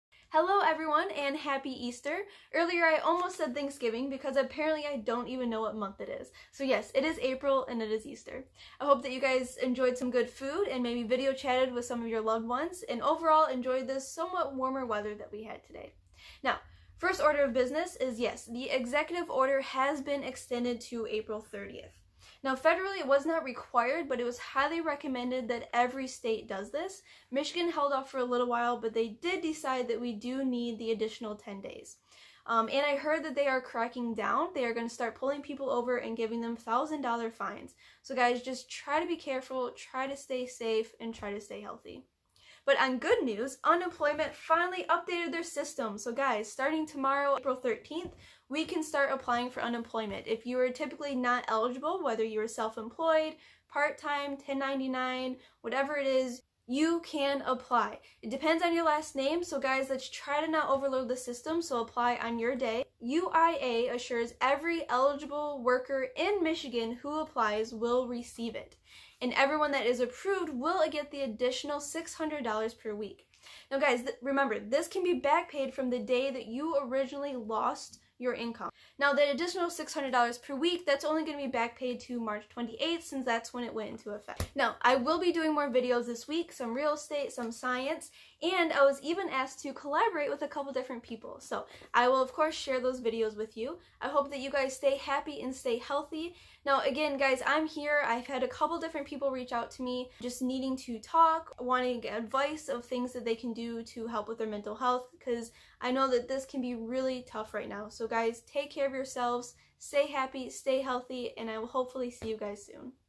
What month is it? April?
Happy Easter. Hope you enjoyed your "social distanced" holiday. Unfortunately the executive order has been extended to April 30th.

Good News! You can now finally apply for unemployment. Anyone whose income was affected by covid, apply on your specified day. Last names starting with A-L apply M W F. Last names starting with M-Z apply T Th Sun. Saturday is for anyone who forgot their day.

More real estate and science videos coming. And even some collaboration videos.